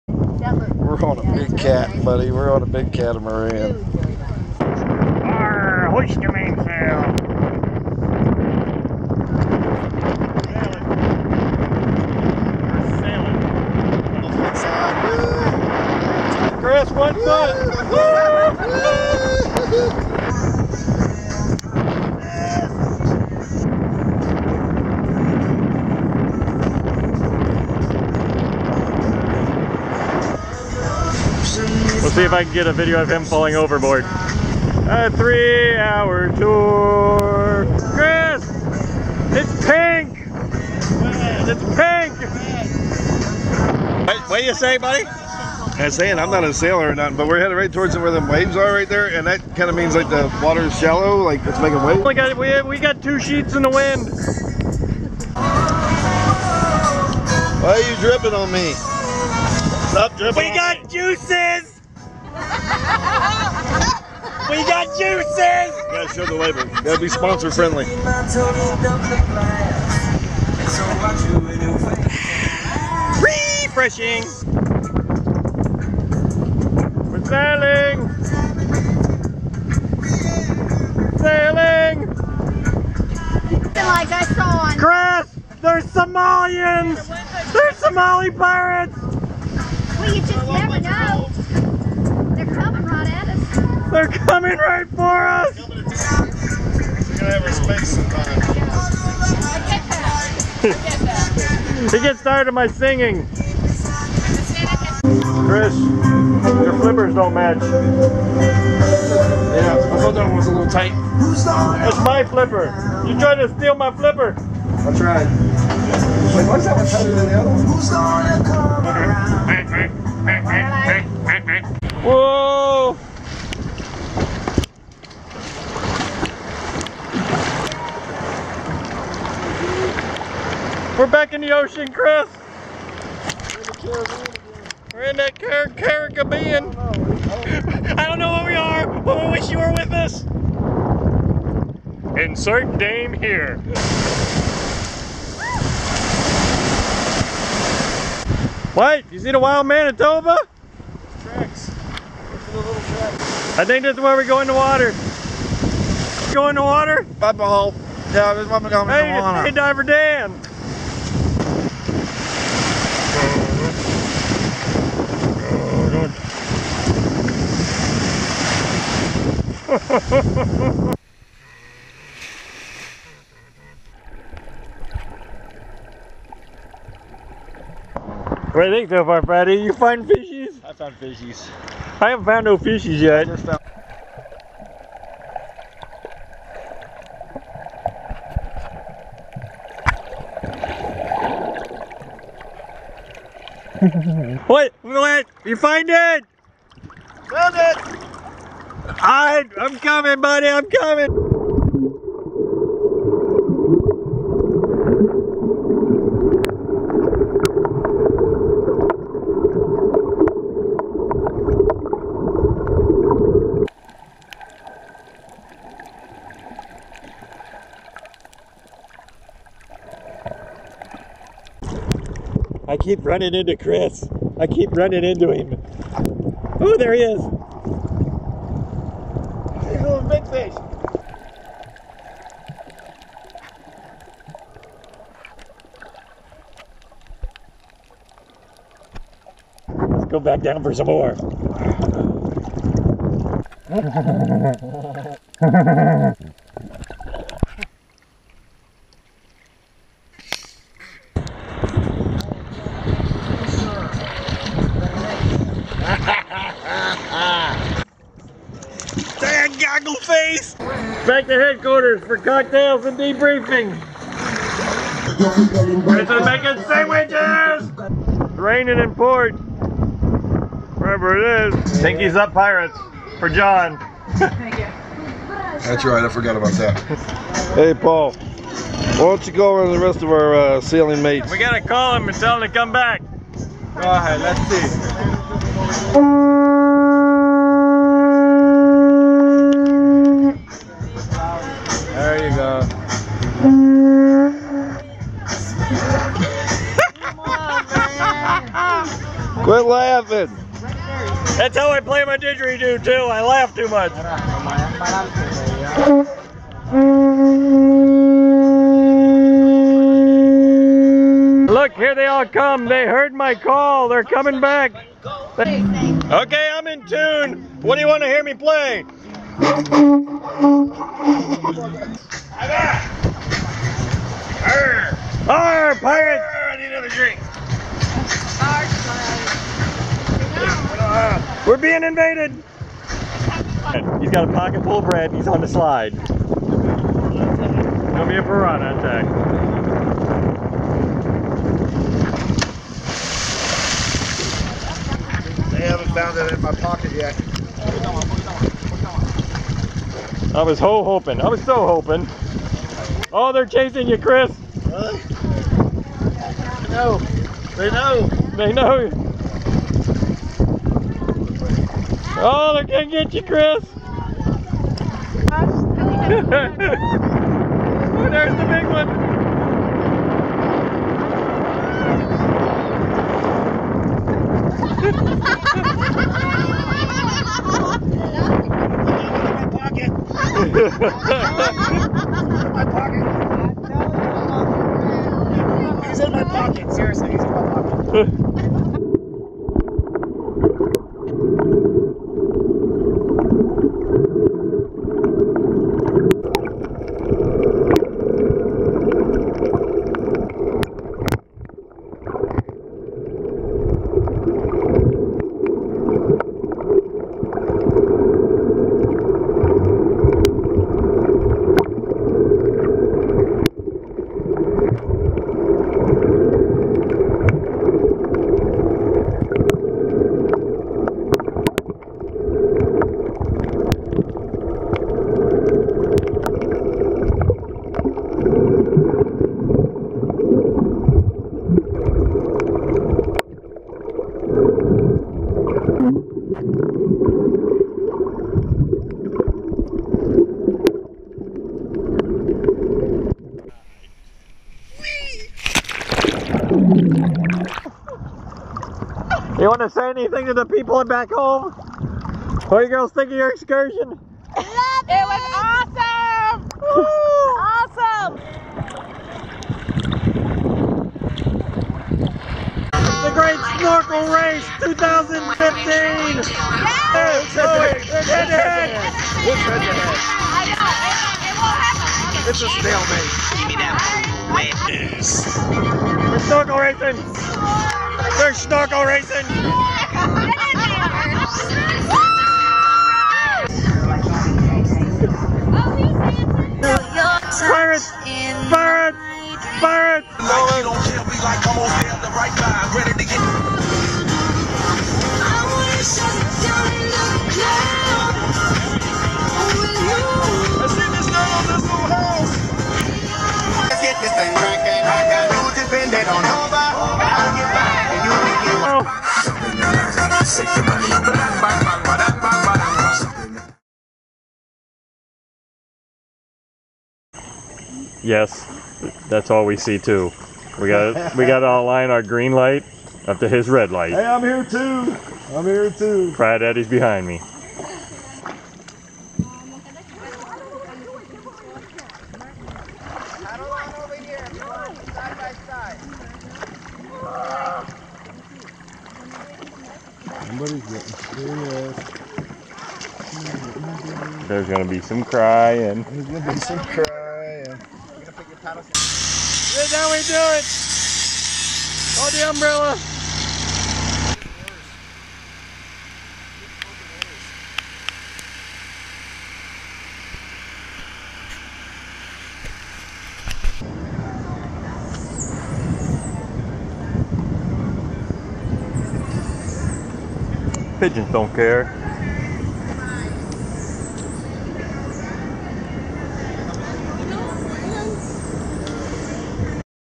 Those Guys TV Puerto Rico 3 Sailing
Those Guys do some Sailing, and snorkling on the end of the Puerto Rico adventure.